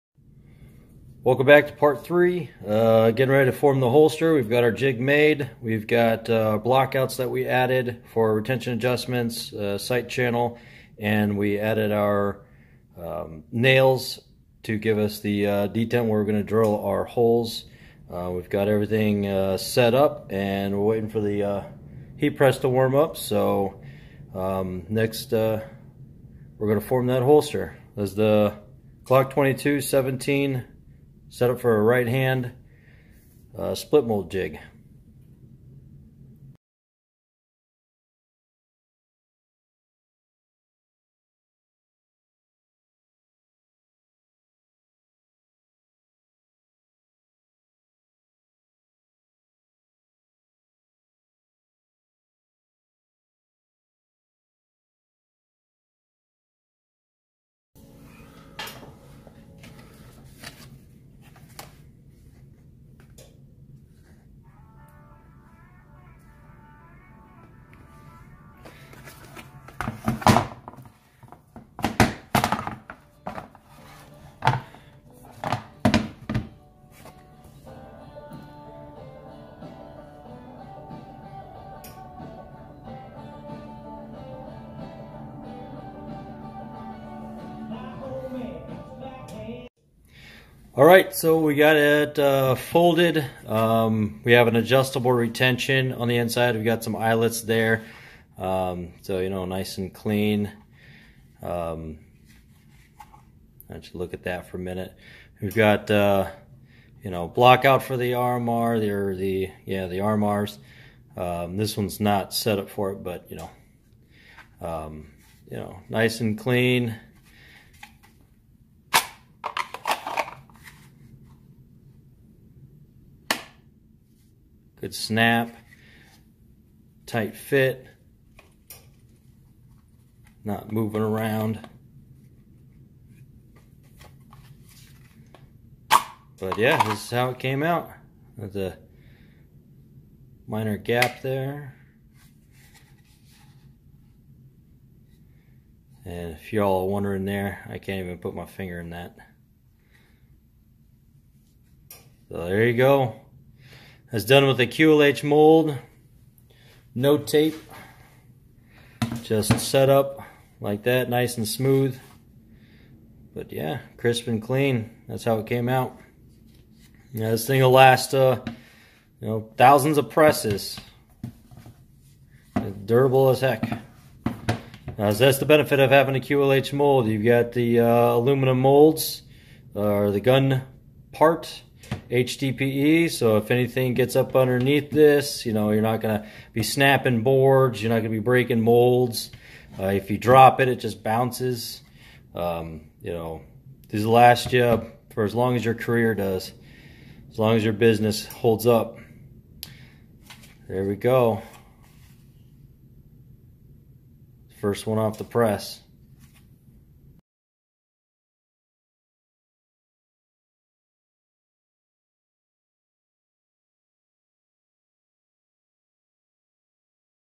G17/22 holster form video 3
In the video we press the first holster with the G17/22 jig that we worked on.